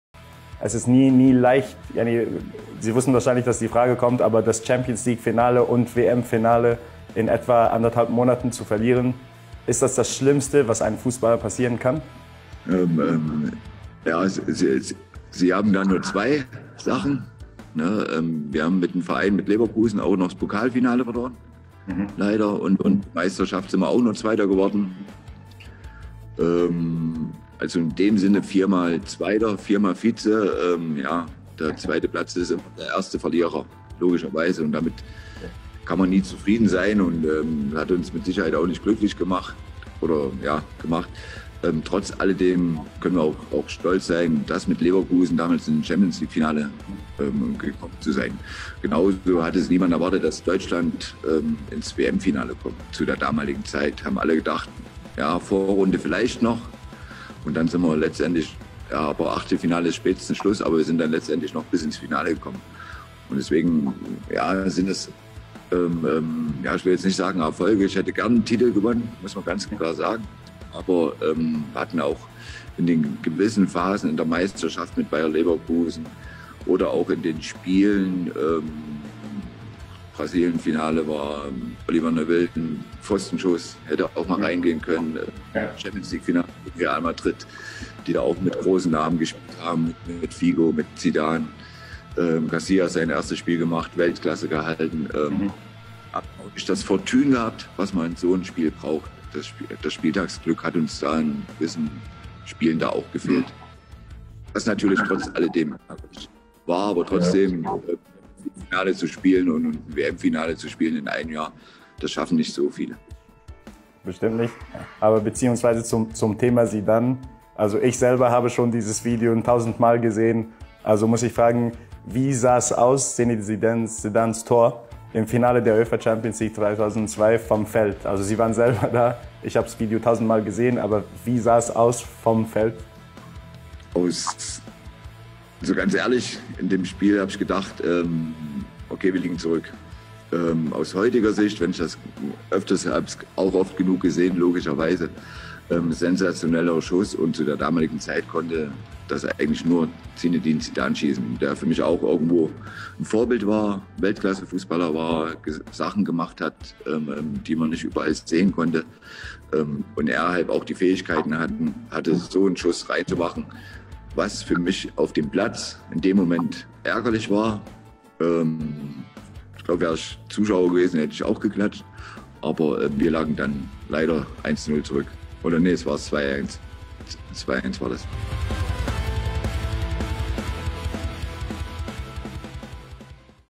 بيرند شنايدر: هدف زيدان في مرمانا كان خرافيا، لو كنت شاهدته من المدرجات لصفقت له
"لعب نهائي دوري الأبطال ونهائي المونديال في عام واحد كان شرفا عضيما"
"هدف زيدان في مرمانا كان خرافيا، لو كنت شاهدته من المدرجات لصفقت له"
بيرند شنايدر لاعب منتخب ألمانيا وباير ليفركوزن يتحدث عن شعور خسارة نهائي المونديال ودوري الأبطال في عام واحد وهد زيدان الأسطوري